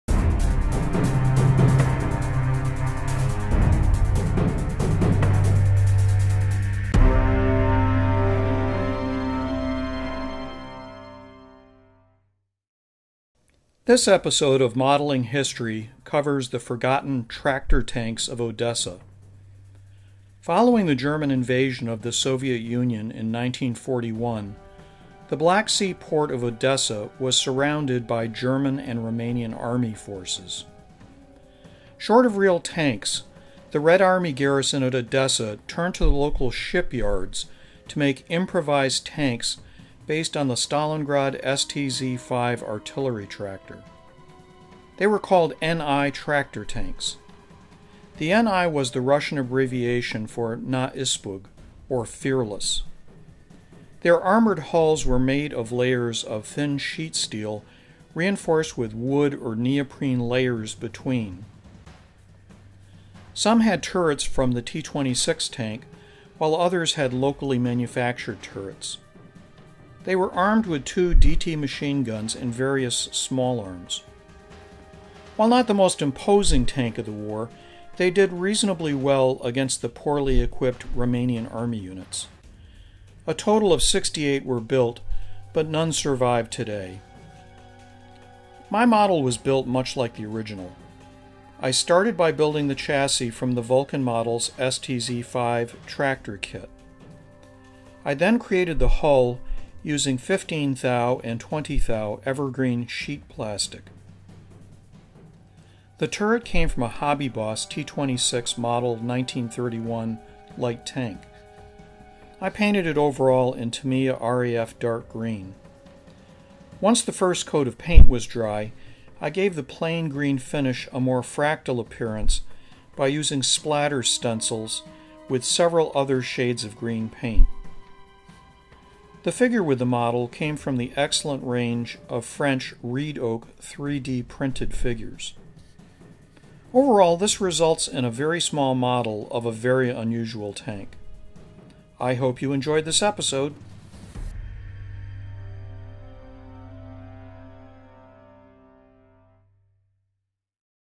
Modeling History: The Fearless Tractor Tanks of Odessa
This episode tells the story of the forgotten Red Army Tractor Tanks that took part in the 1941 battles around the port city of Odessa. It then explains how a 1:35 scale model of the NI Tractor Tank was created starting with a Vulcan Models STZ-5 tractor and adding a scratch-built hull.